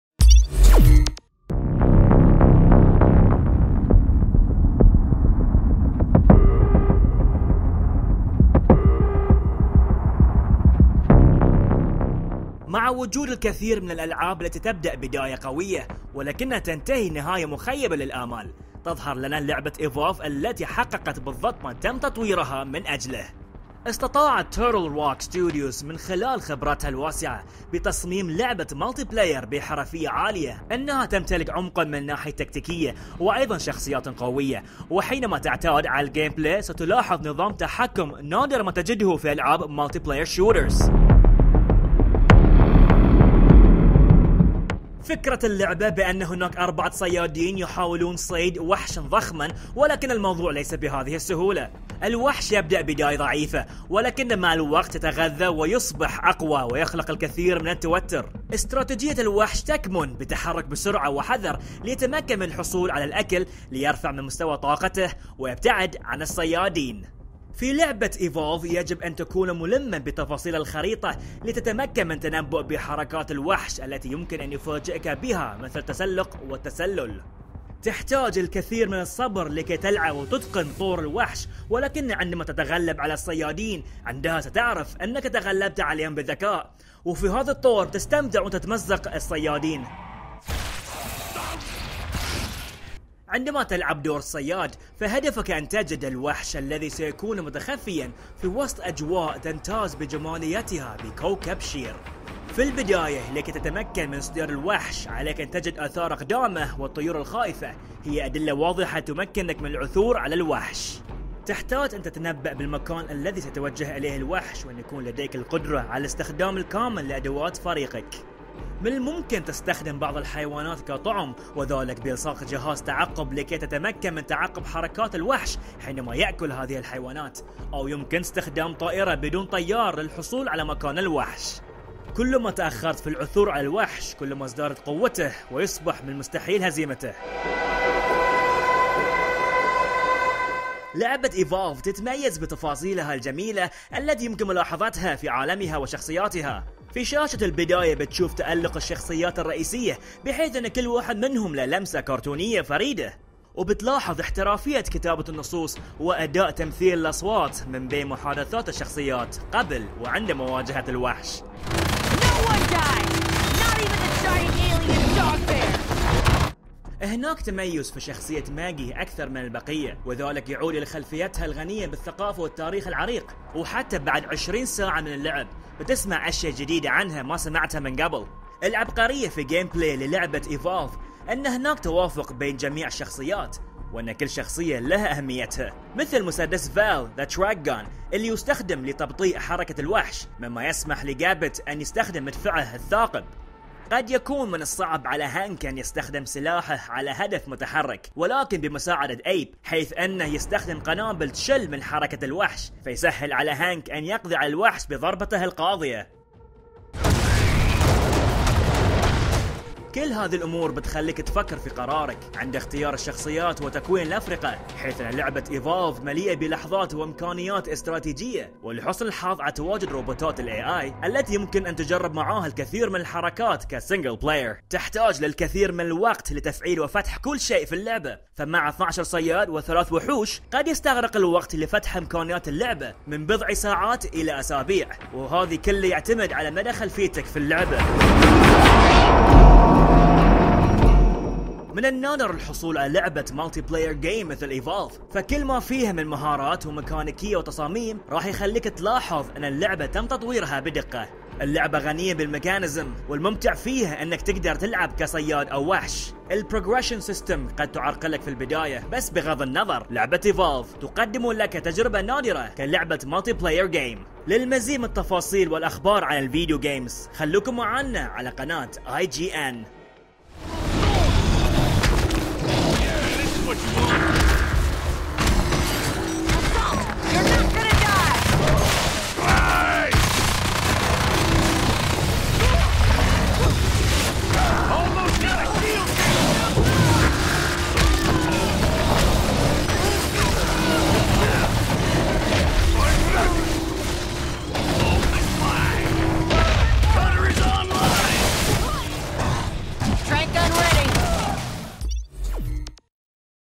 مراجعة Evolve
عمق تكتيكي، شخصيات مثيرة وانماط لعب فريدة من نوعها، Evolve تقدم مستوى مميز نادرا ما يتواجد في الألعاب المعتمدة على الأطوار الجماعية.